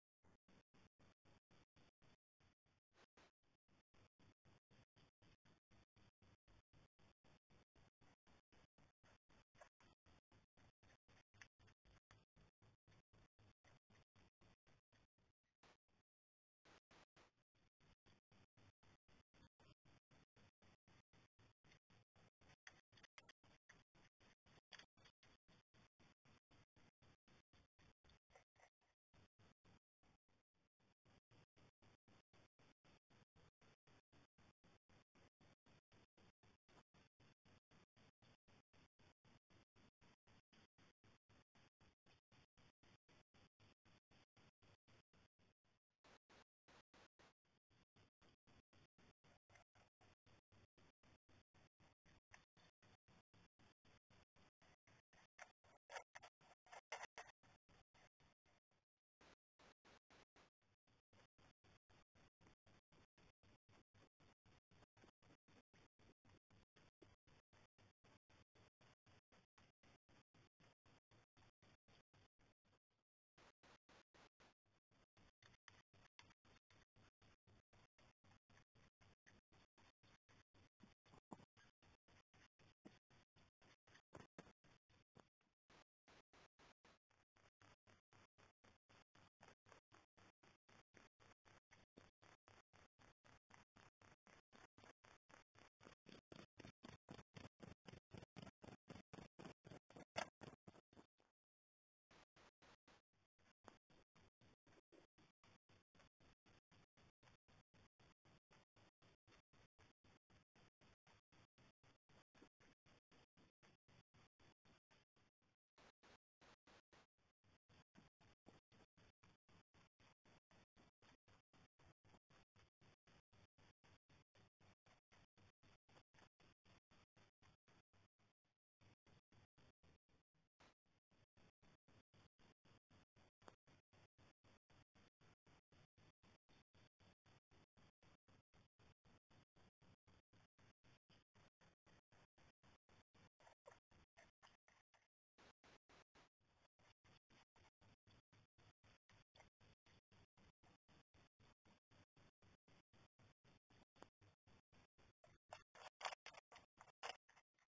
Hogvlog 25.10.2018
Hedgehogs coming to feed in my garden